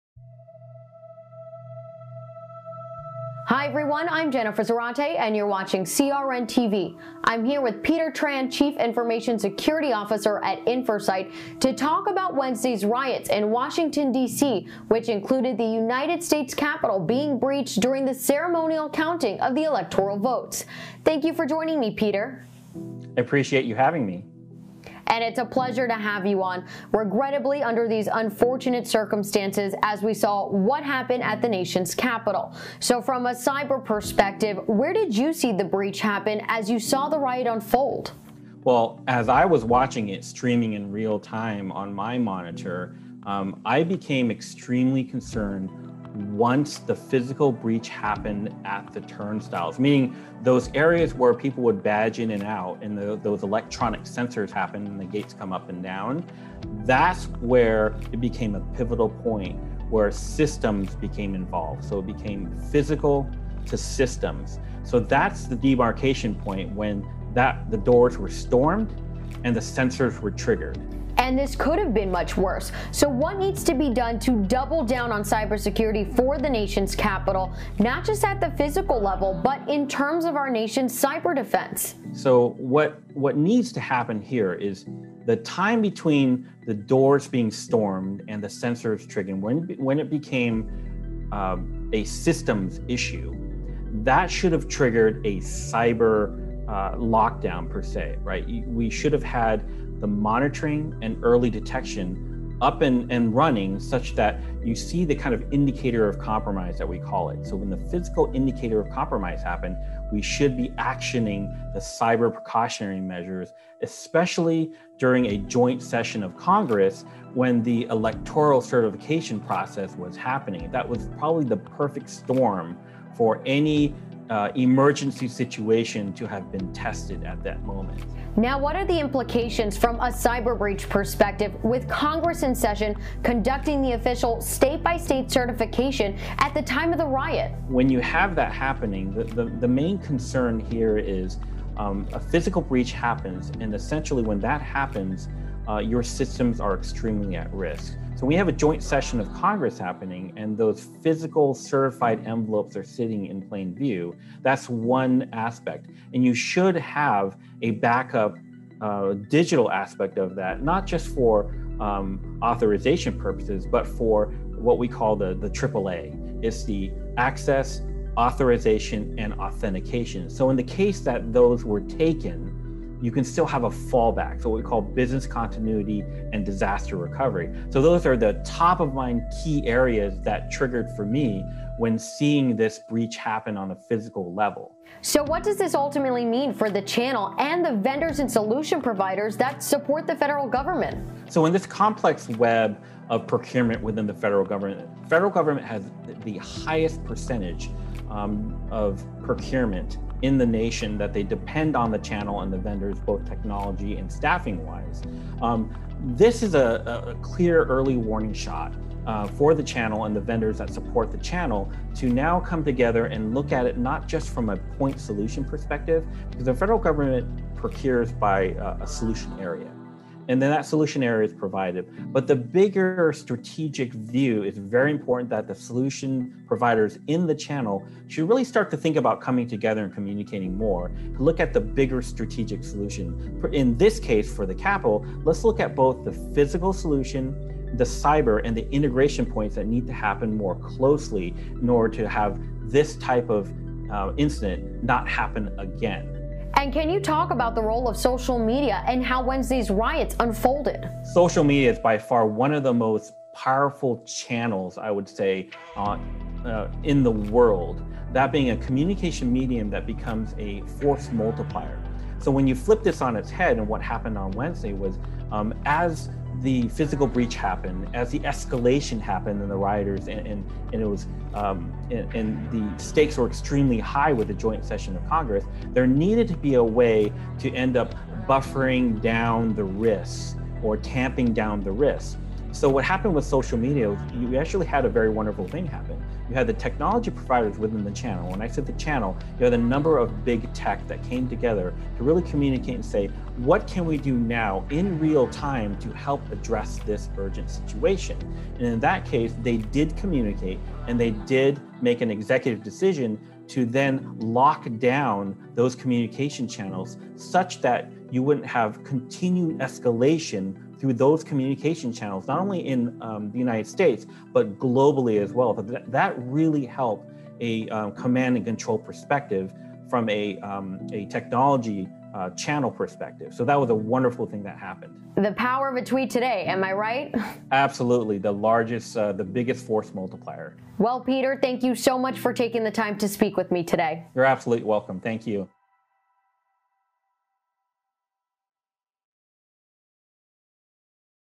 (CRNTV) - US Capitol Riots Aftermath - Need To Double Down On Nations Cyber Defense
The US Capital riots aftermath leave many unanswered questions one of which is. "what was the cybersecurity impact when the rioters breached the gates and guards to storm the capital?"

With over $590 Billion in federal government spending on private contracting across all of its functions, I sat down with Jennifer Zarate The Channel Company CRNTV and discussed where the failures may be, critical security areas of consideration and what government channel partners across the nation should be thinking about.